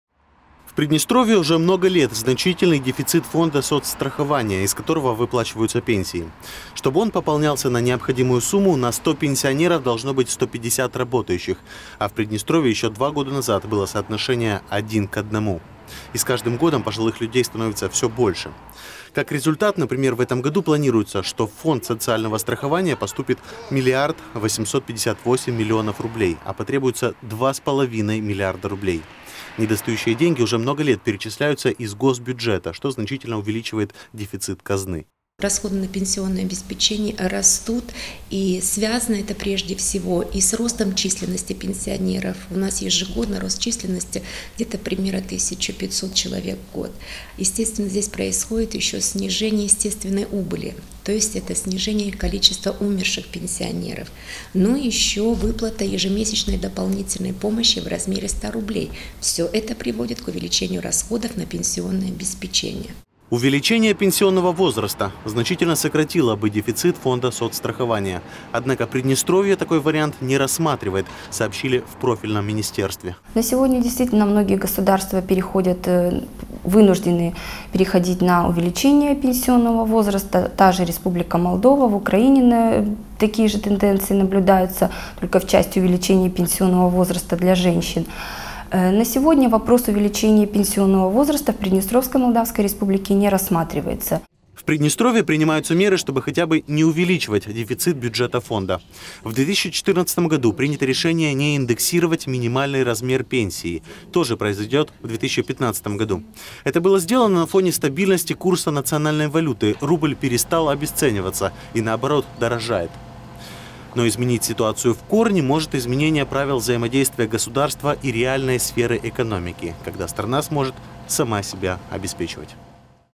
Повышения пенсионного возраста в республике не будет
В Приднестровье не планируют увеличивать пенсионный возраст, хотя соседние государства пошли на такие меры. Об этом сообщили в министерстве по социальной защите и труду.